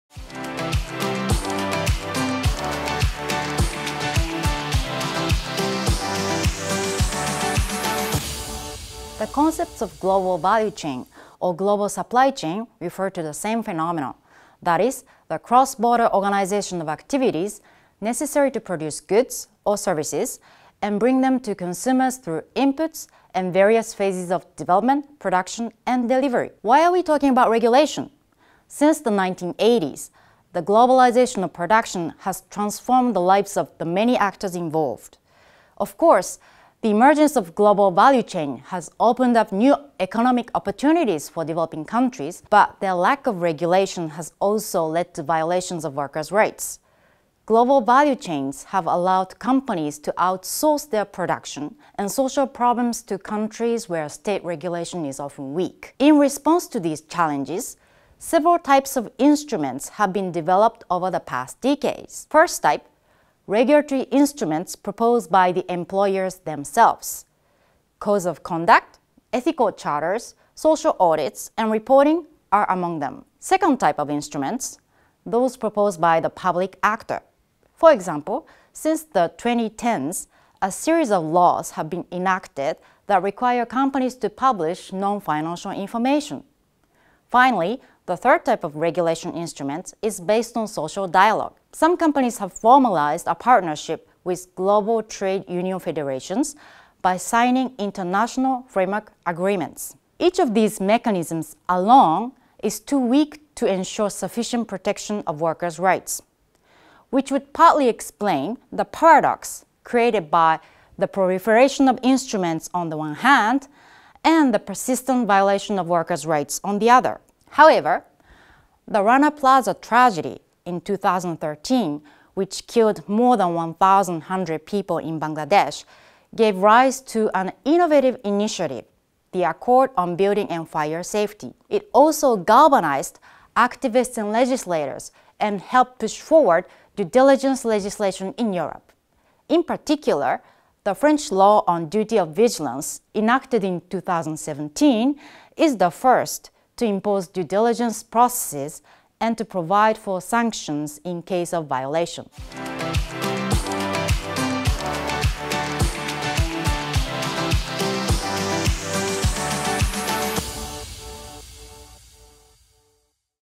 What is Regulation of global value chain ?
La chaîne globale de valeur, cela veut dire « l’organisation transfrontalière des activités nécessaires pour produire des biens ou des services et les amener aux consommateurs par le biais d’intrants et de diverses phases de développement, de production et de livraison ».
Si l’émergence des chaînes globale de valeur a dessiné de nouvelles opportunités économiques pour les pays en voie de développement, leur absence de régulation a également conduit à des violations des droits des travailleurs. Pour répondre aux enjeux de régulation des chaînes globales de valeur, une diversité d’instruments a été développée au cours des dernières décennies. Chacun de ces mécanismes est à lui seul trop faible pour assurer une protection suffisante des droits des travailleurs, ce qui expliquerait en partie le paradoxe engendré par la prolifération d’instruments d’une part, et les violations persistantes des droits des travailleurs d’autre part.

Auteur : OKA Chikako
Titre : Maître de conférences
Institution : IAE Gustave Eiffel, Université Paris-Est Créteil